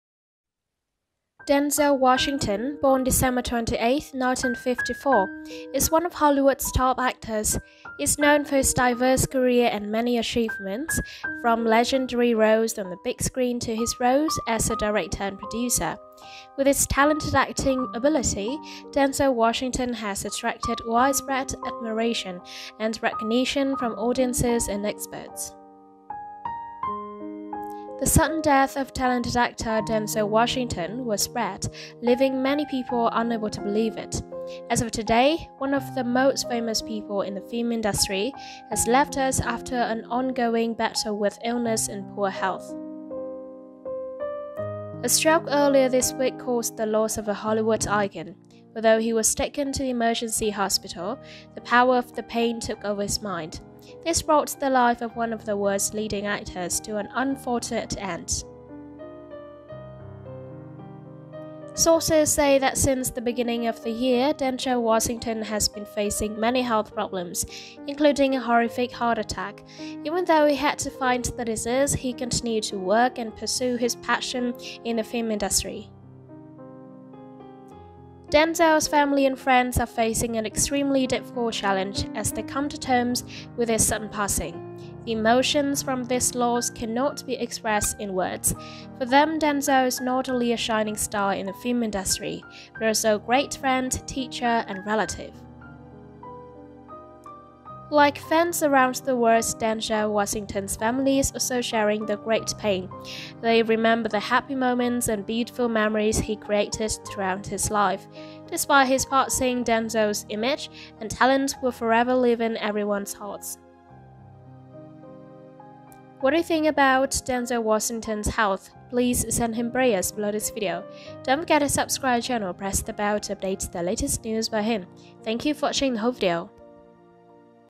R.I.P. Denzel Washington Just Died In The Hospital, Expected Soon Family Prepare To Say bye.
Denzel Hayes Washington Jr. (born December 28, 1954) is an American actor and filmmaker. He has been described as an actor who reconfigured "the concept of classic movie stardom".[1] Throughout his career spanning over four decades, Washington has received numerous accolades, including a Tony Award, two Academy Awards, three Golden Globe Awards and two Silver Bears.[2] In 2016, he received the Cecil B. DeMille Lifetime Achievement Award, and in 2020, The New York Times named him the greatest actor of the 21st century.[3] In 2022, Washington received the Presidential Medal of Freedom bestowed upon him by President Joe Biden.